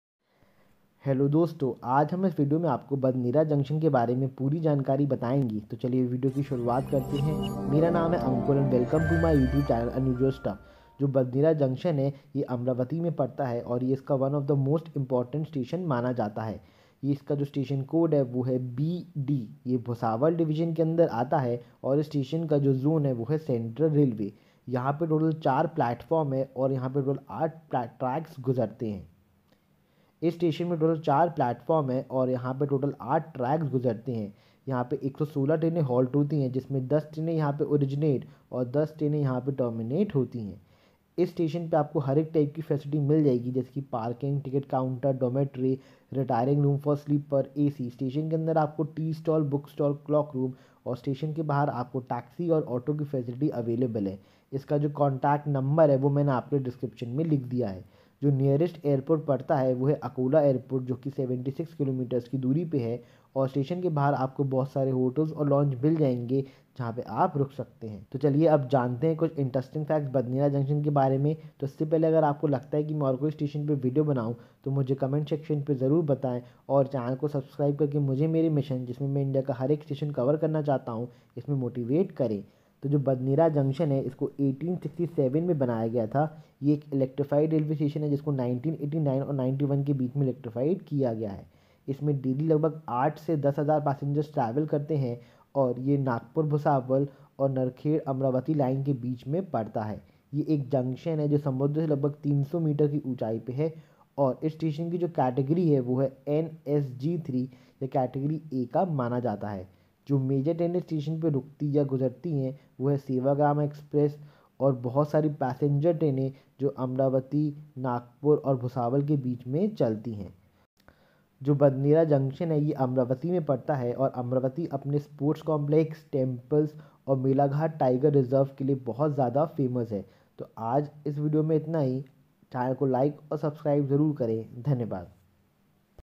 Badnera Junction, Amravati
Badnera Jn Railway station is situated in Amravati, Maharashtra. Station code of Badnera Jn is BD. Here are some trains that are passing through Badnera Jn railway station like Maharashtra Exp, Maharashtra Exp, Navjeevan Exp, Navajivan Exp, Tpty Ami Sup Exp, Ami Tpty Sup Exp, Howrah Mail